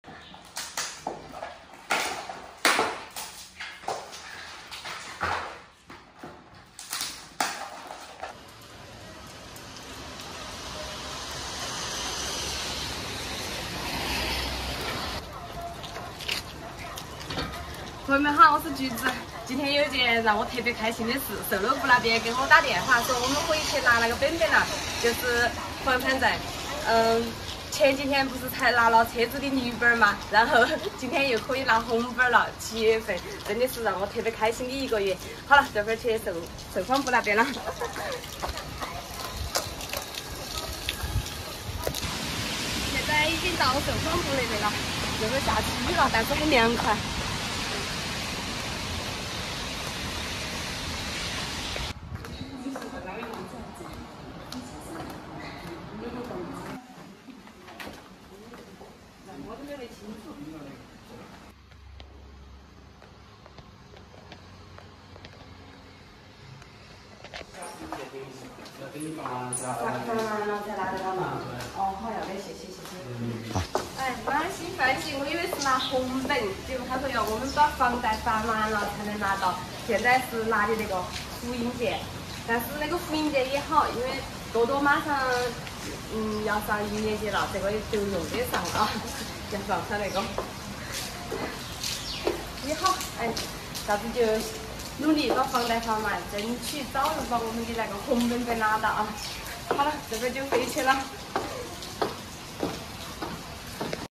菊子满心欢喜去售房部拿房产证，结果却出了意外，还得努力啊
菊子老爸今天過生日，好多親戚都來了，家裡好熱鬧